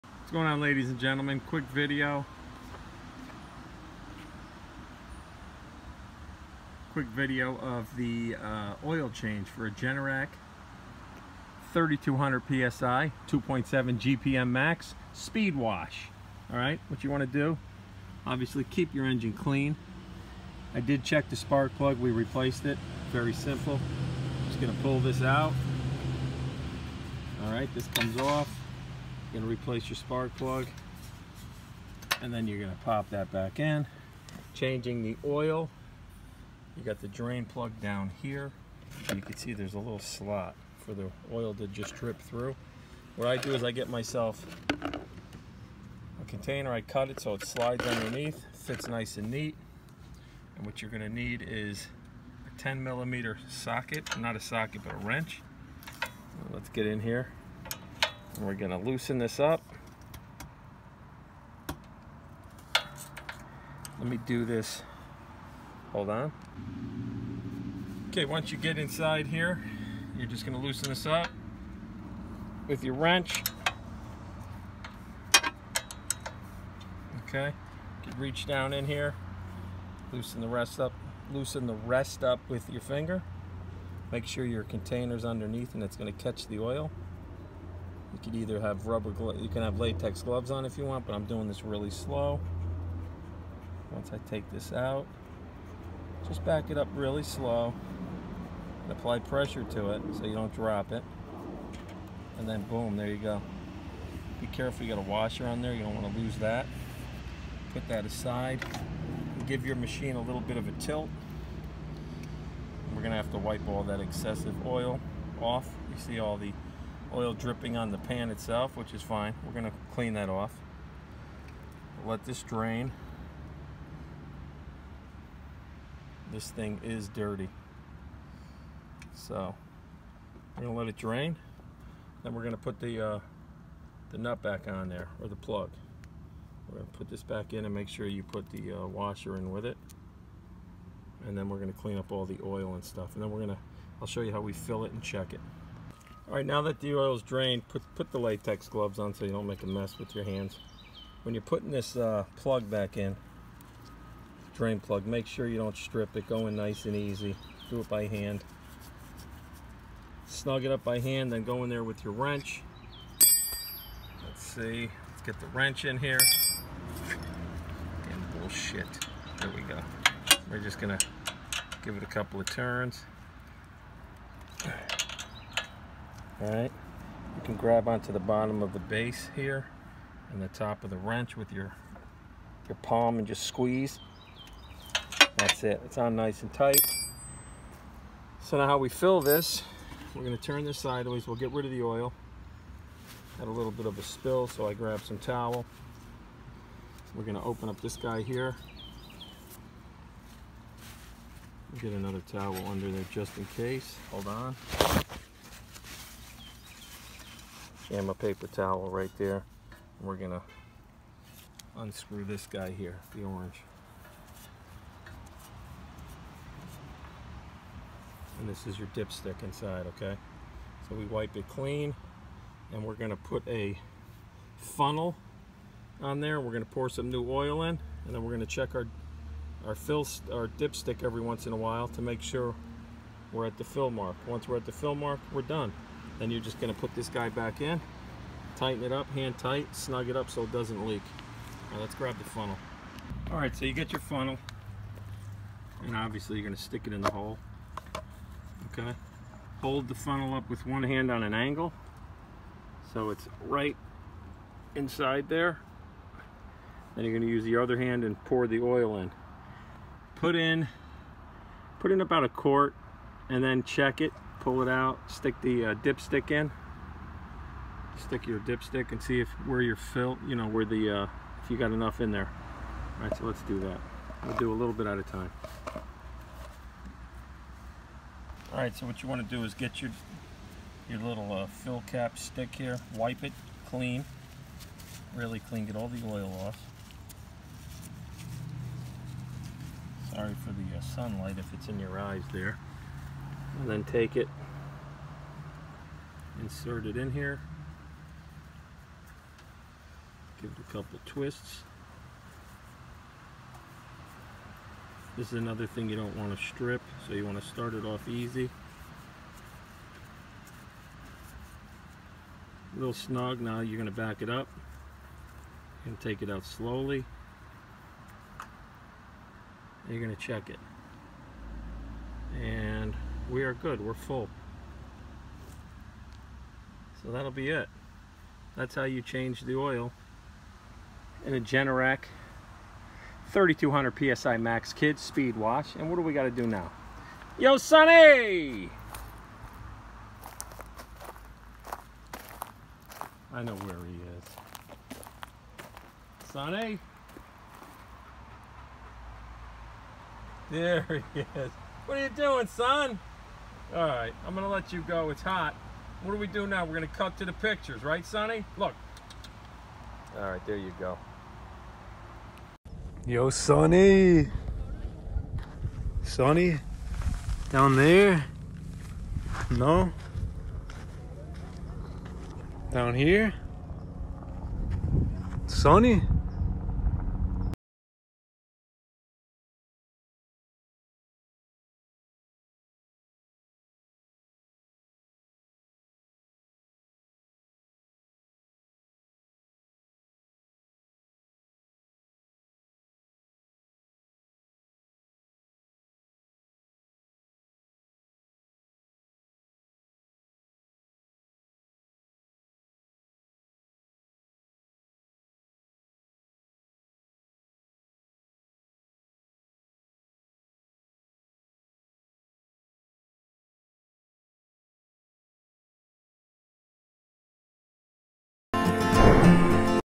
Generac 3200 psi speedwash oil change
Check out this quick video on changing the oil in the Generac 3200 psi speedwash🇺🇸